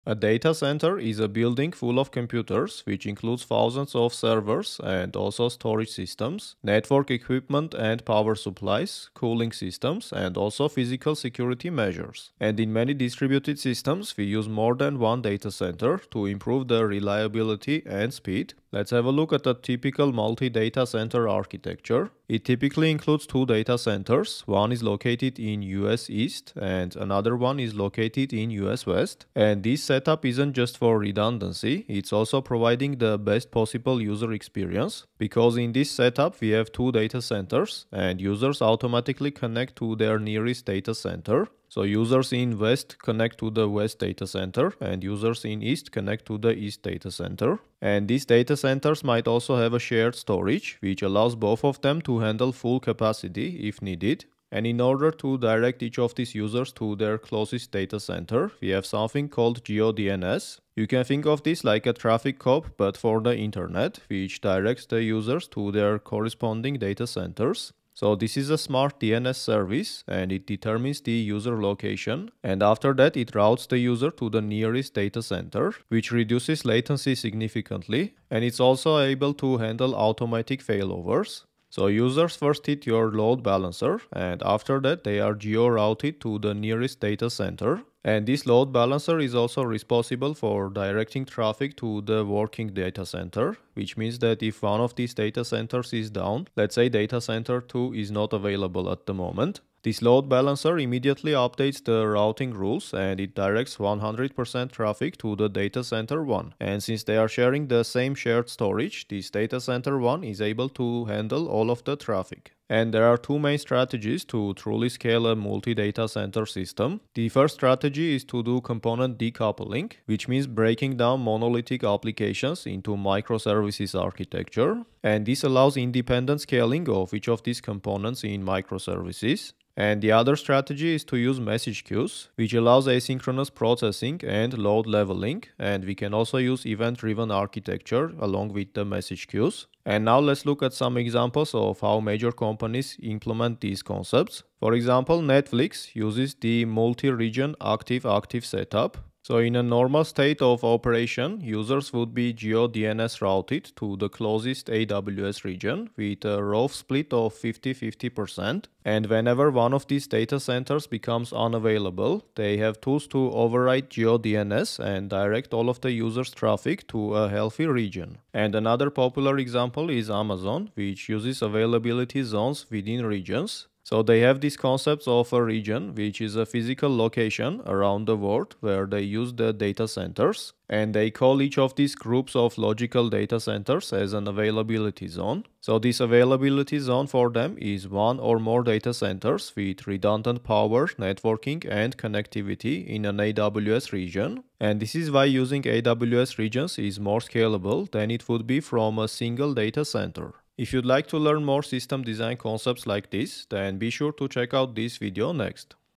Data Centers in System Design
Learn how data centers work in system design, covering multi-center setups, geo-routing, data synchronization, and failover strategies.